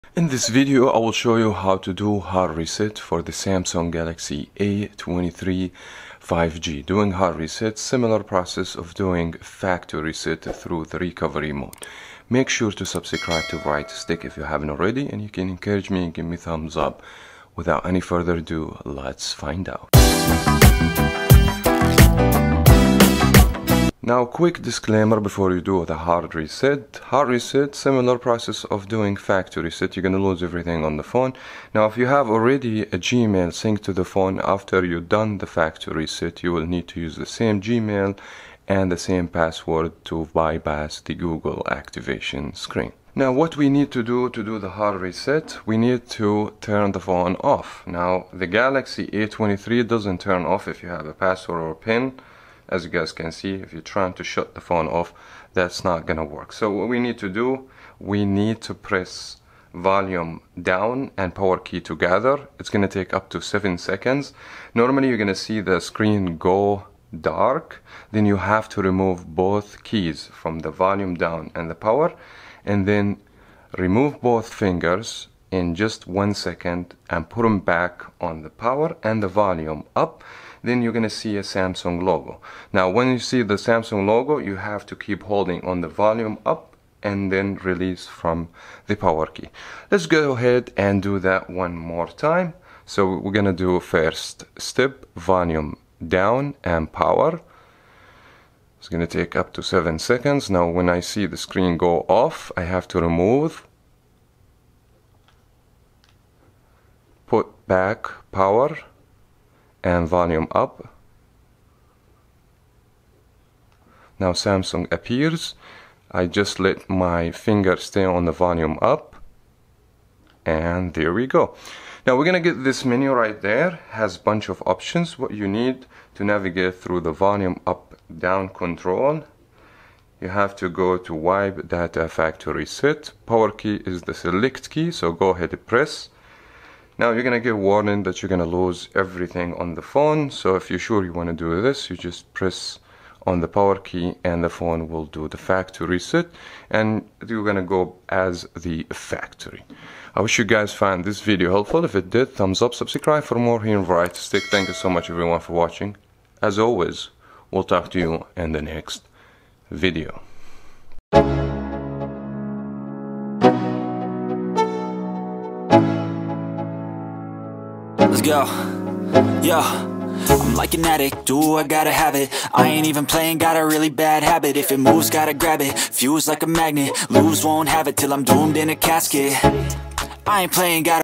Samsung Galaxy A23 5G How to Hard Reset Removing PIN, Password, Fingerprint pattern
Samsung Galaxy A23 How to Hard Reset  Removing PIN, Password, Fingerprint pattern